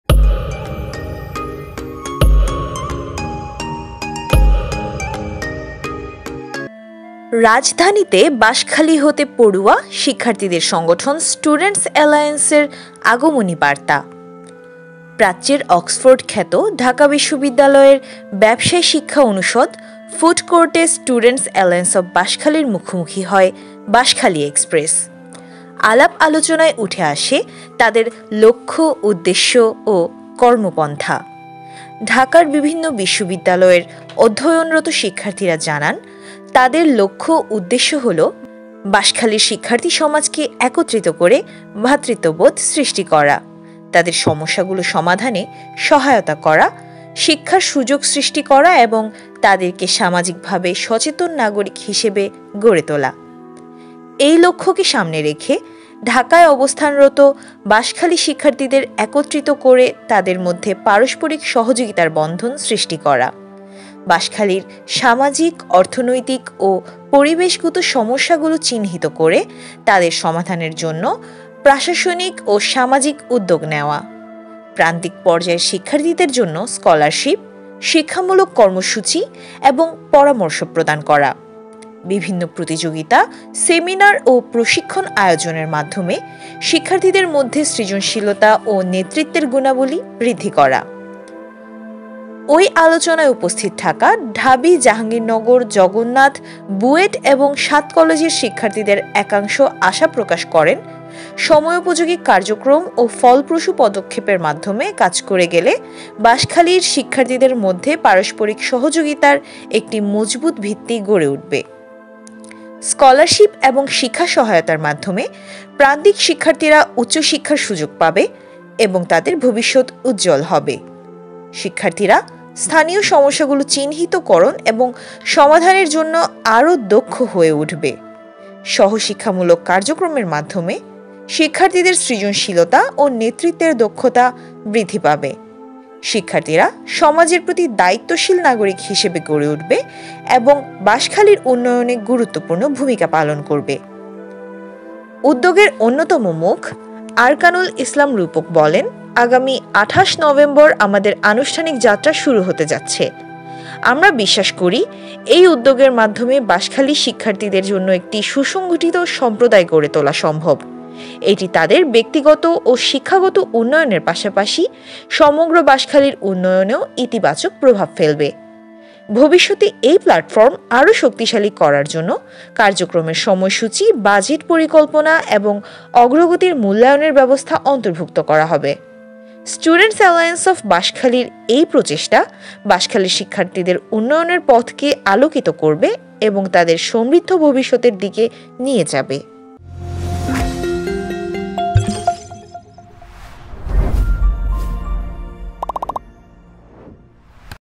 সংগঠন সংবাদ | 100| স্টুডেন্টস এলায়েন্স অব বাঁশখালীর আগমনী বার্তা
রাজধানীতে বাঁশখালী হতে পড়ুয়া শিক্ষার্থীদের সংগঠন স্টুডেন্টস এলায়েন্স'র আগমনী বার্তা

প্রাচ্যের অক্সফোর্ড খ্যাত ঢাকা বিশ্ববিদ্যালয়ের  ব্যবসায় শিক্ষা অনুষদ, ফুডকোর্টে  স্টুডেন্টস এলায়েন্স অব বাঁশখালীর মুখোমুখি হয় বাঁশখালী এক্সপ্রেস। আলাপ আলোচনায় উঠে আসে তাঁদের লক্ষ্য উদ্দেশ্য ও কর্মপন্থা। ঢাকার বিভিন্ন বিশ্ববিদ্যালয়ের অধ্যয়নরত শিক্ষার্থীরা জানান তাঁদের লক্ষ্য উদ্দেশ্য হল বাঁশখালীর শিক্ষার্থী সমাজকে একত্রিত করে ভ্রাতৃত্ববোধ সৃষ্টি করা, তাদের সমস্যাগুলো সমাধানে সহায়তা করা, শিক্ষার সুযোগ সৃষ্টি করা এবং তাদেরকে সামাজিকভাবে সচেতন নাগরিক হিসেবে গড়ে তোলা। এই লক্ষ্যকে সামনে রেখে ঢাকায় অবস্থানরত বাঁশখালী শিক্ষার্থীদের একত্রিত করে তাদের মধ্যে পারস্পরিক সহযোগিতার বন্ধন সৃষ্টি করা। বাঁশখালীর সামাজিক, অর্থনৈতিক ও পরিবেশগত সমস্যাগুলো চিহ্নিত করে তাদের সমাধানের জন্য প্রশাসনিক ও সামাজিক উদ্যোগ নেওয়া।
প্রান্তিক পর্যায়ের শিক্ষার্থীদের জন্য স্কলারশিপ, শিক্ষামূলক কর্মসূচি এবং পরামর্শ প্রদান করা। বিভিন্ন প্রতিযোগিতা, সেমিনার ও প্রশিক্ষণ আয়োজনের মাধ্যমে শিক্ষার্থীদের মধ্যে সৃজনশীলতা ও নেতৃত্বের গুণাবলী বৃদ্ধি করা।

ঐ আলোচনায় উপস্থিত থাকা ঢাবি, জাহাঙ্গীর নগর, জগন্নাথ, বুয়েট এবং সাত কলেজের শিক্ষার্থীদের একাংশ আশা প্রকাশ করেন সময়োপযোগী কার্যক্রম ও ফলপ্রসূ
পদক্ষেপের মাধ্যমে কাজ করে গেলে বাঁশখালীর শিক্ষার্থীদের মধ্যে পারস্পরিক সহযোগিতার একটি মজবুত ভিত্তি গড়ে উঠবে। স্কলারশিপ এবং শিক্ষা সহায়তার মাধ্যমে প্রান্তিক শিক্ষার্থীরা উচ্চশিক্ষার সুযোগ পাবে এবং তাদের ভবিষ্যৎ উজ্জ্বল হবে।
শিক্ষার্থীরা স্থানীয় সমস্যাগুলো চিহ্নিত এবং সমাধানের জন্য আরও দক্ষ হয়ে উঠবে।সহশিক্ষামূলক কার্যক্রমের মাধ্যমে শিক্ষার্থীদের সৃজনশীলতা ও নেতৃত্বের দক্ষতা বৃদ্ধি পাবে। শিক্ষার্থীরা সমাজের প্রতি দায়িত্বশীল নাগরিক হিসেবে গড়ে উঠবে এবং বাঁশখালীর উন্নয়নে গুরুত্বপূর্ণ ভূমিকা পালন করবে।

উদ্যোগের অন্যতম মুখ আরকানুল ইসলাম রূপক বলেন,' আগামী ২৮ নভেম্বর আমাদের আনুষ্ঠানিক যাত্রা শুরু হতে যাচ্ছে। আমরা বিশ্বাস করি এই উদ্যোগের মাধ্যমে বাঁশখালী শিক্ষার্থীদের জন্য একটি সুসংগঠিত সম্প্রদায় গড়ে তোলা সম্ভব। এটি তাদের ব্যক্তিগত ও শিক্ষাগত উন্নয়নের পাশাপাশি সমগ্র বাঁশখালীর উন্নয়নেও ইতিবাচক প্রভাব ফেলবে। ভবিষ্যতে এই প্ল্যাটফর্ম আরও শক্তিশালী করার জন্য কার্যক্রমের সময়সূচি, বাজেট পরিকল্পনা, এবং অগ্রগতির মূল্যায়নের ব্যবস্থা অন্তর্ভুক্ত করা হবে।

Students Alliance of Banskhali-এর এই প্রচেষ্টা বাঁশখালীর শিক্ষার্থীদের উন্নয়নের পথকে আলোকিত করবে এবং তাদেরকে সমৃদ্ধ ভবিষ্যতের দিকে নিয়ে যাবে।